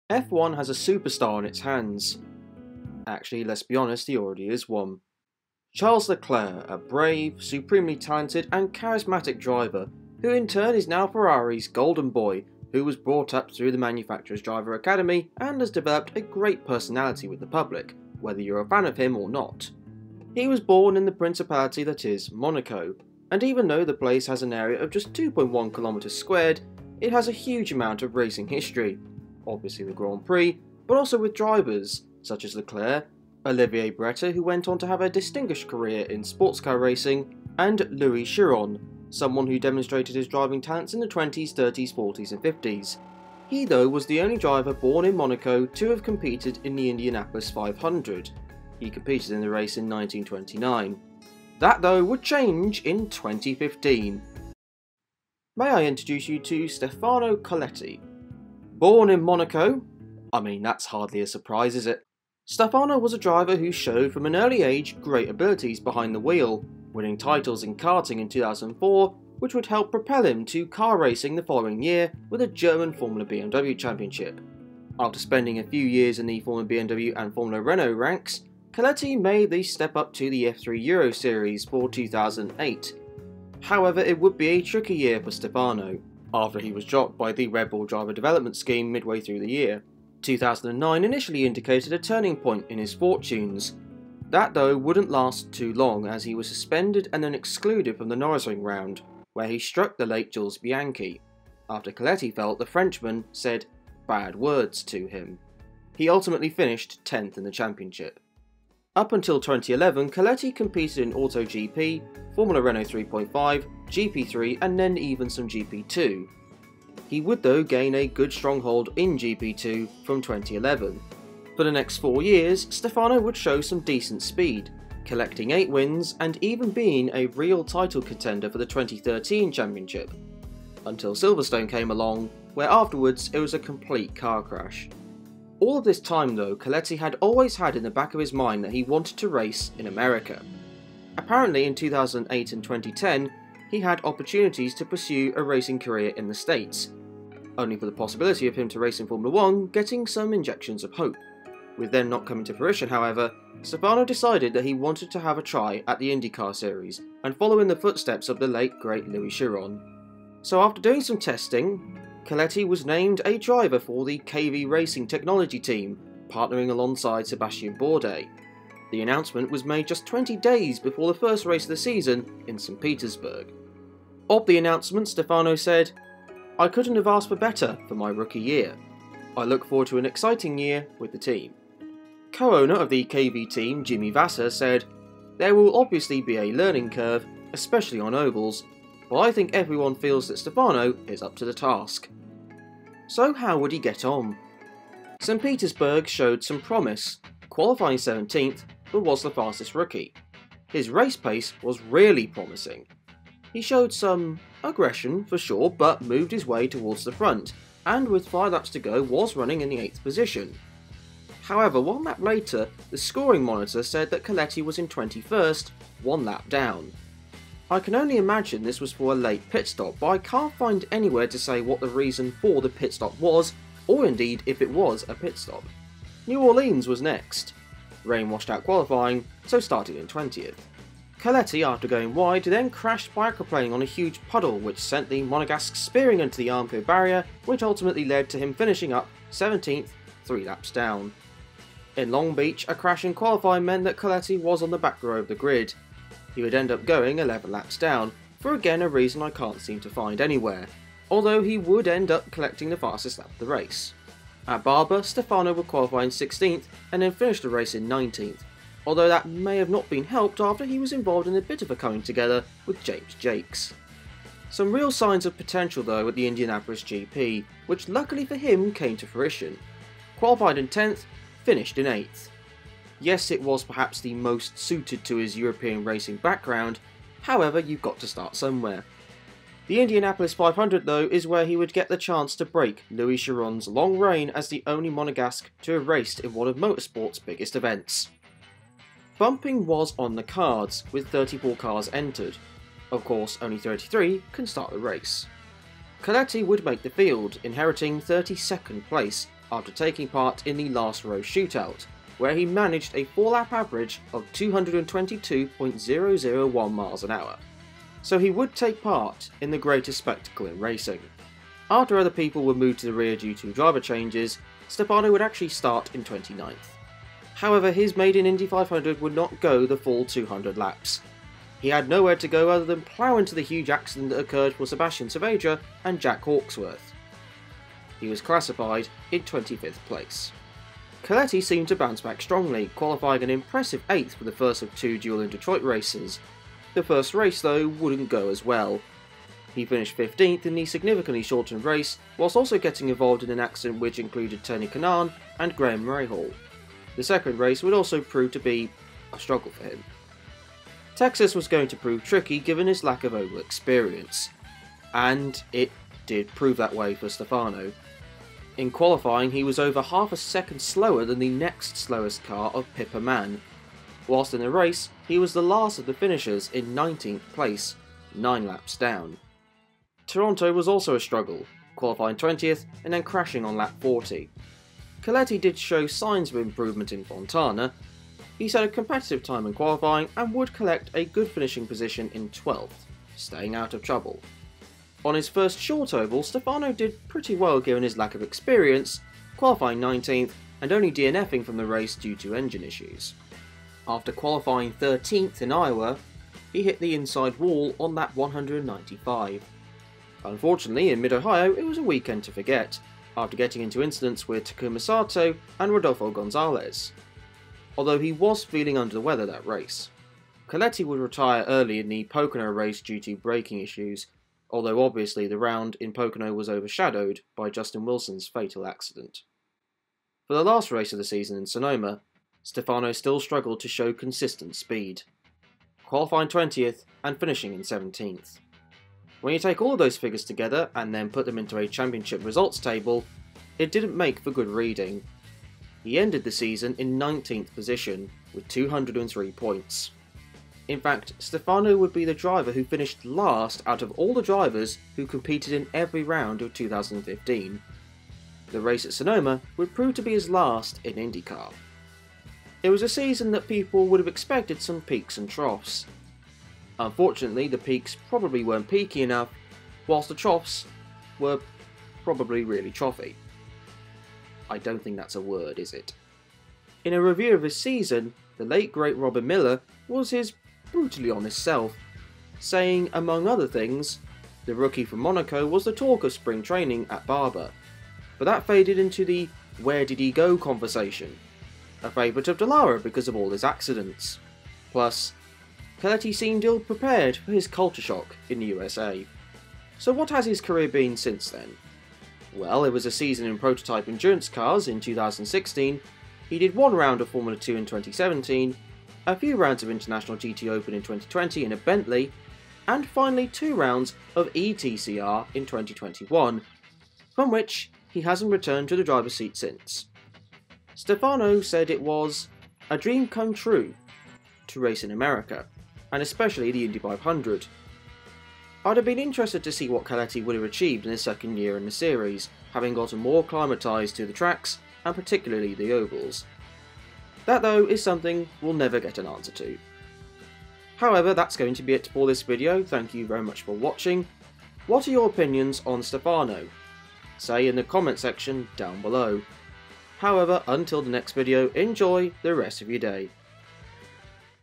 Stefano Coletti: USA's Charles Leclerc
Monaco, despite it's small land area, has a huge amount of history when it comes to motorsport. However, one driver always seems to be forgotten about, Stefano Coletti, who tried to make an impact on the racing world stateside.